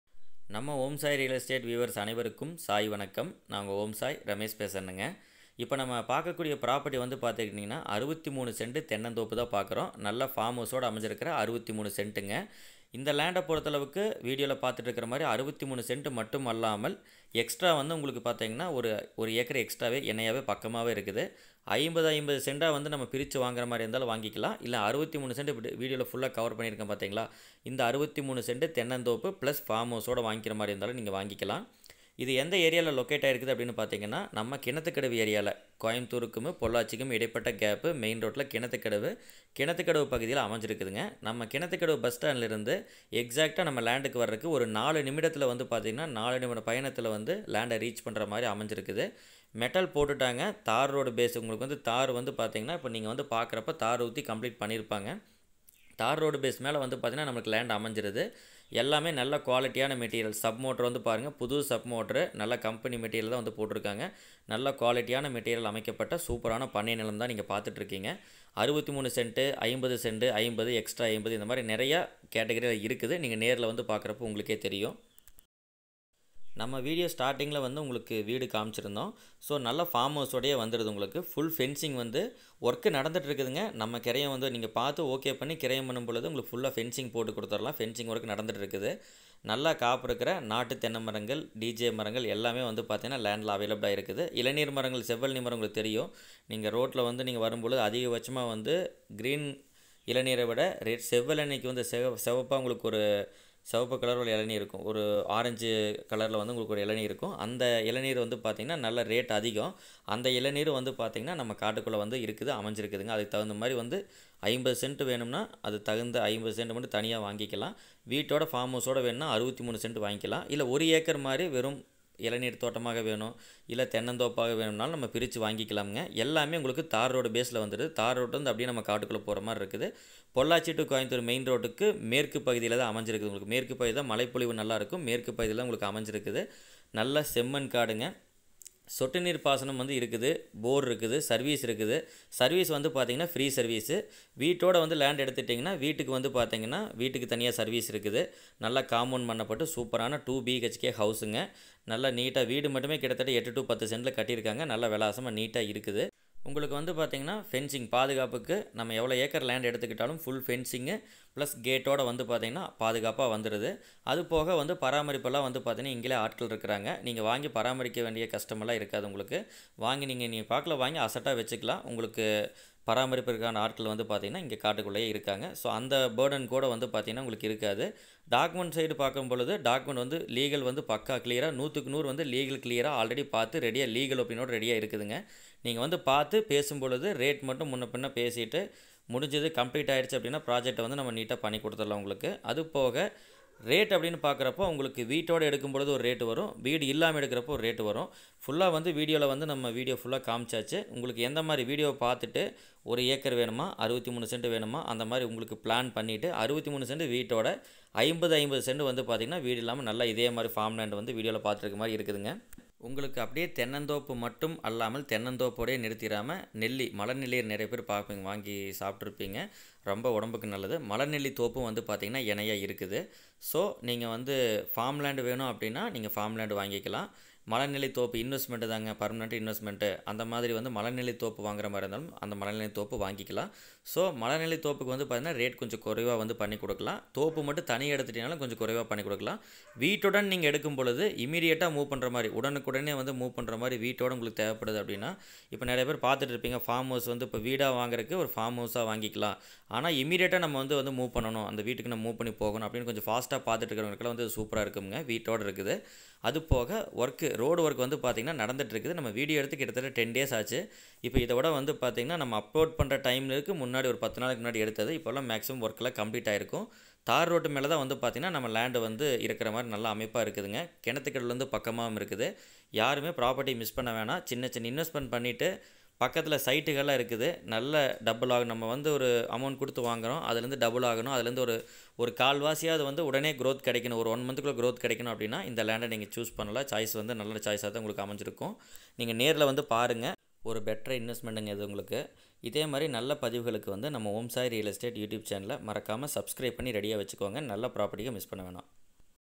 1025) அழகான ஹவுஸ் உடன் கிணத்துக்கடவு ஏரியாவில் 50 சென்ட் , 1 ஏக்கர் தென்னந்தோப்பு விற்பனைக்கு
50 cents to 1 acre  farm land with beautiful farm house for sale in Kinathukadavu

Om Sai Real Estate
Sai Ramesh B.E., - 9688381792